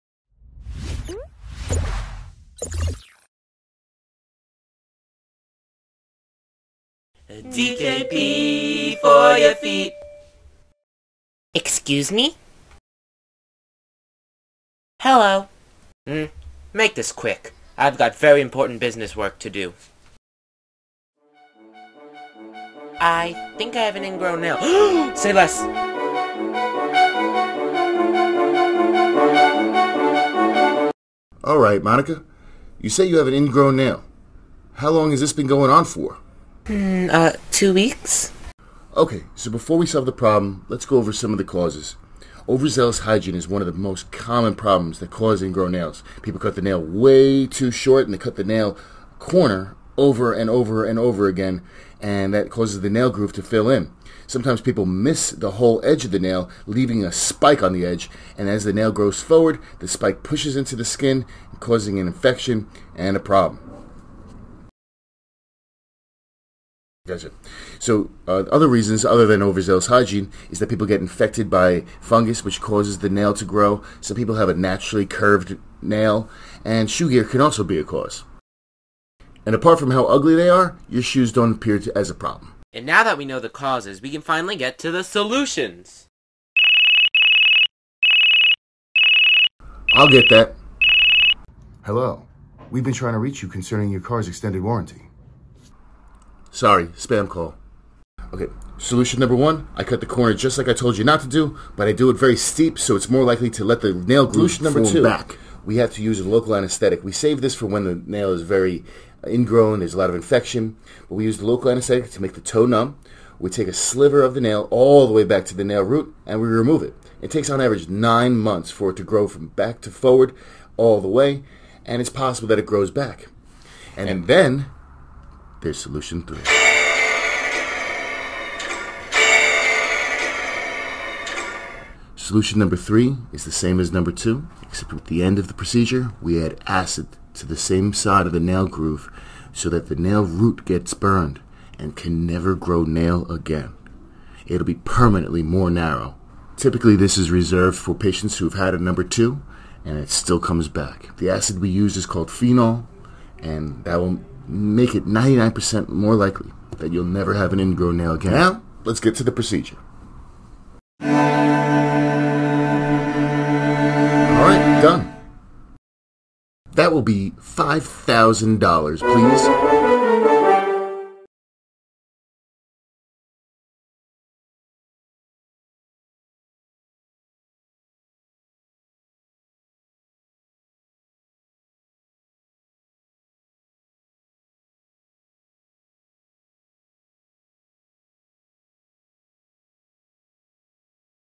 Ingrown Toenail Animation! - Podiatry Explained by Dr. Roger Kilfoil
Ingrown toenails are a common condition in which the corner or side of a toenail grows into the soft flesh. The result is pain, inflamed skin, swelling and, sometimes, an infection. Ingrown toenails usually affect the big toe.

Often you can take care of ingrown toenails on your own. If the pain is severe or spreading, your health care provider can take steps to relieve your discomfort and help you avoid complications of ingrown toenails.

If you have diabetes or another condition that causes poor blood flow to your feet, you're at greater risk of complications of ingrown toenails.